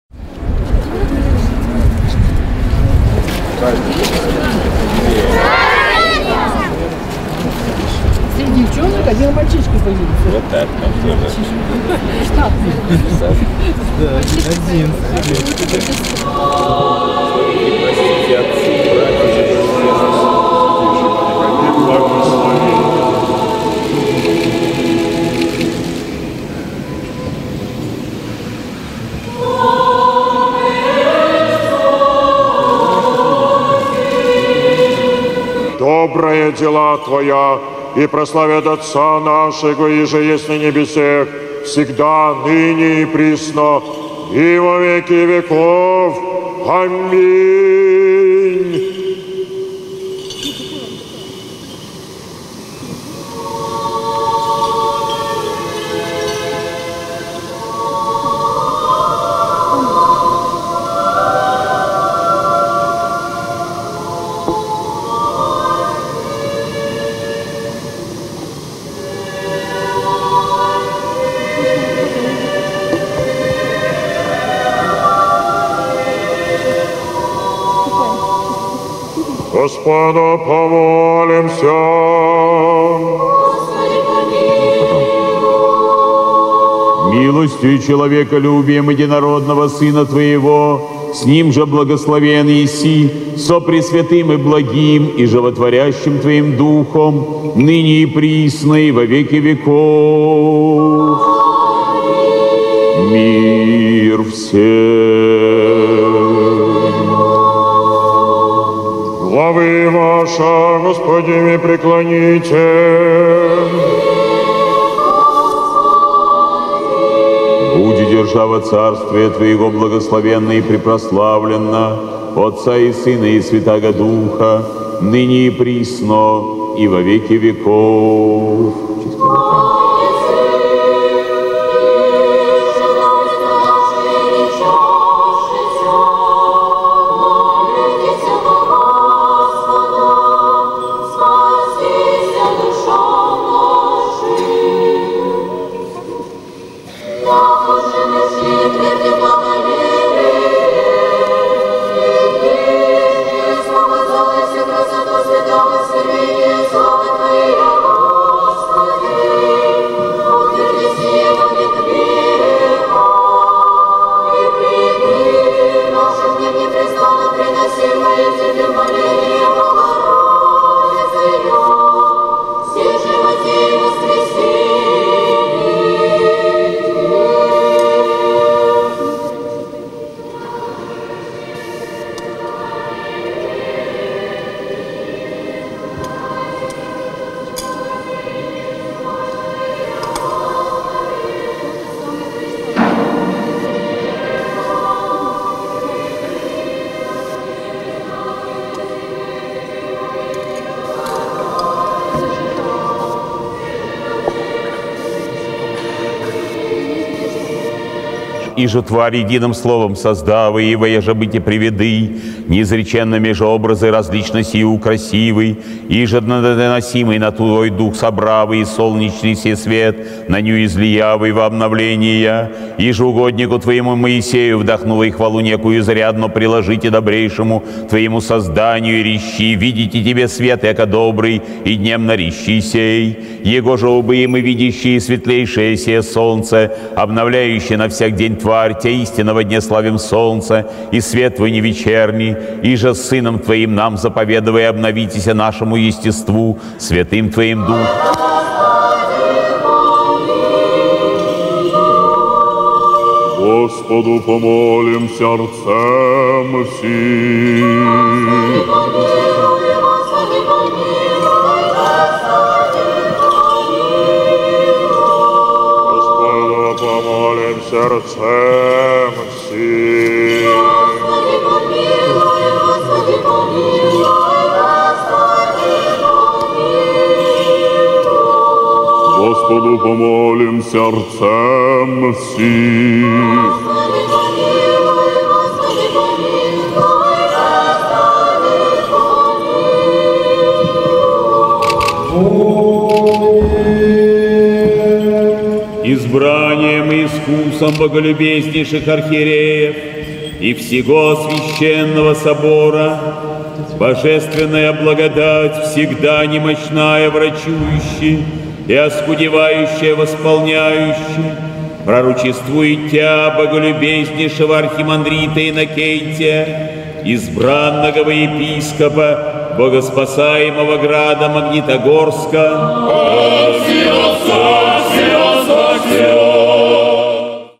Патриарх Кирилл совершил богослужение в Хотькове
11 октября 2012 года, в день памяти прпп. схимонаха Кирилла и схимонахини Марии, родителей прп. Сергия Радонежского, Святейший Патриарх Московский и всея Руси Кирилл совершил чин великого освящения возрожденного Никольского собора Покровского ставропигиального женского монастыря в Хотькове (Московская область) и Божественную литургию в новоосвященном храме. За Литургией была совершена хиротония архимандрита Иннокентия (Васецкого) во епископа Магнитогорского и Верхнеуральского.